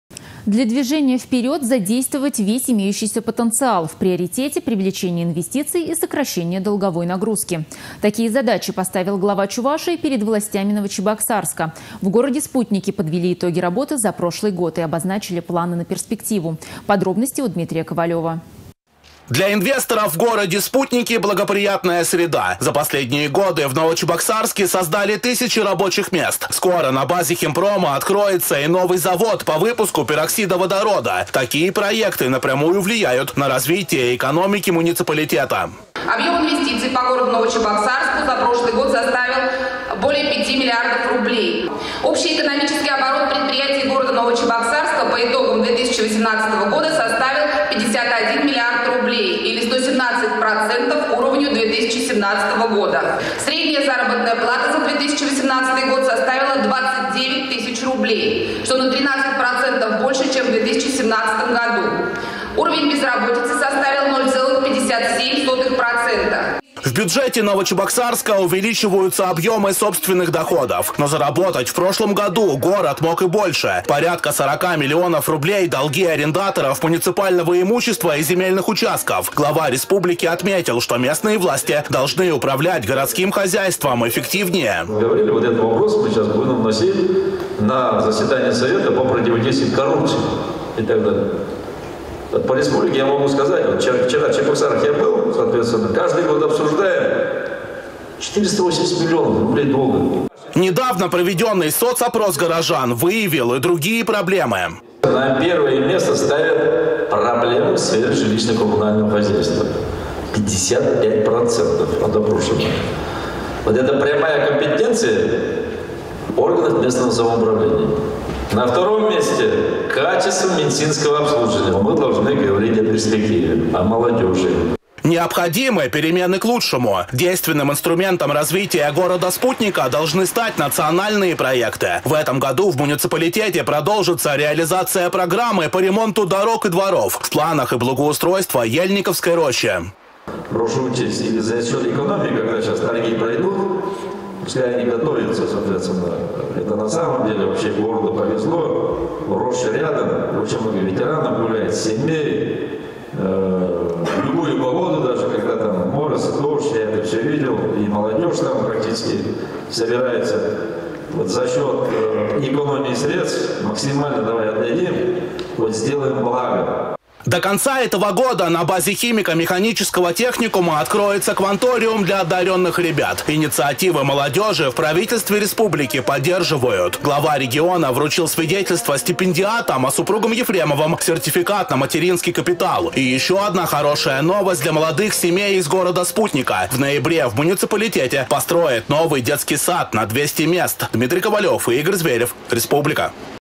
Для движения вперёд - задействовать весь имеющийся потенциал.
В приоритете - привлечение инвестиций и сокращение долговой нагрузки. Такие задачи поставил Глава Чувашии перед властями Новочебоксарска. В городе-спутнике подвели итоги работы за прошлый год и обозначили планы на перспективу